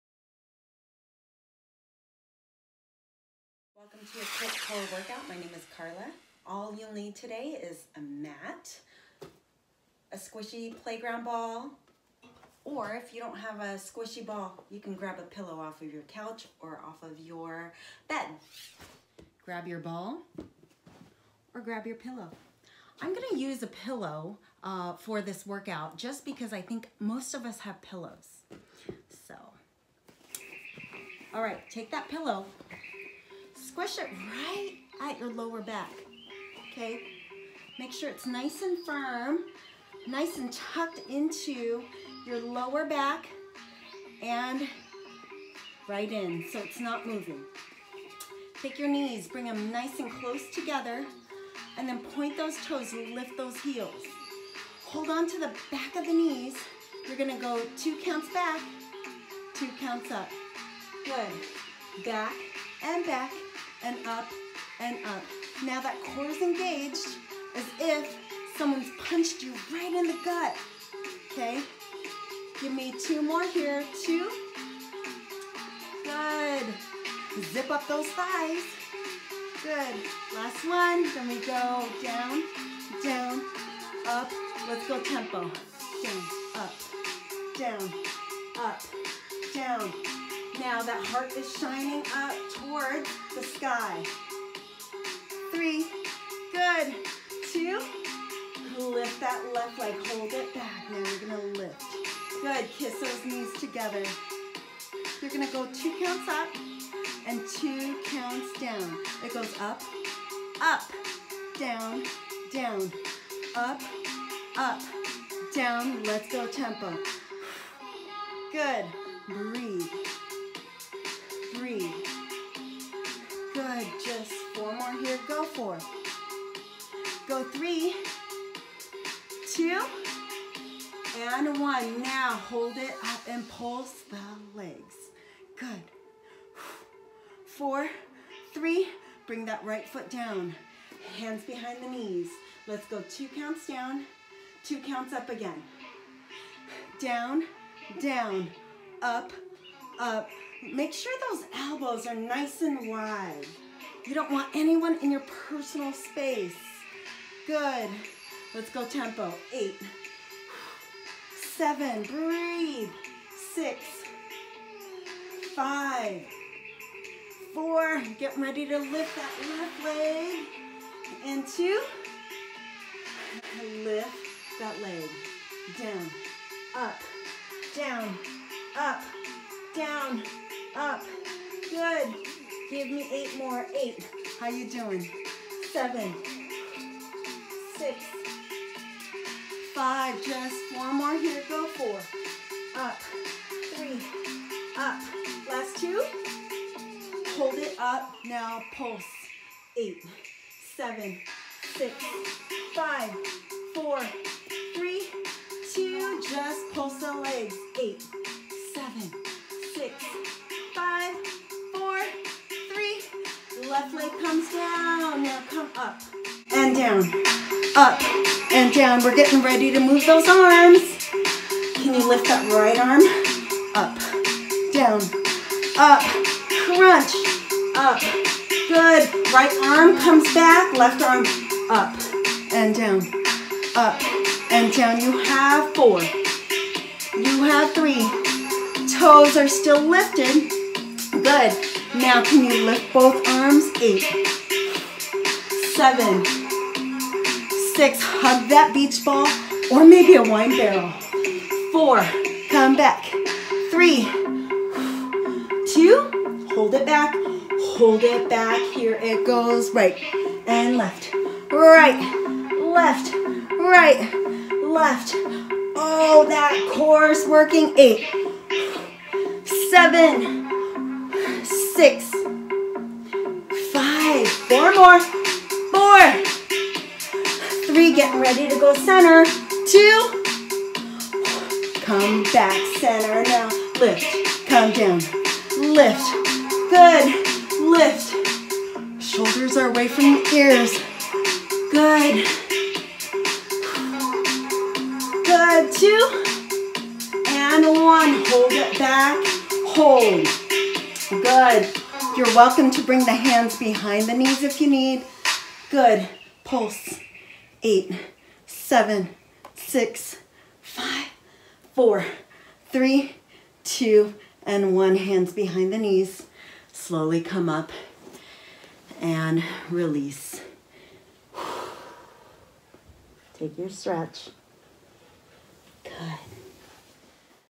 QUICK CORE WORKOUT!! 💪🏽
Grab a ball or pillow and join Carla as she takes you through a 5 minute core burn 🔥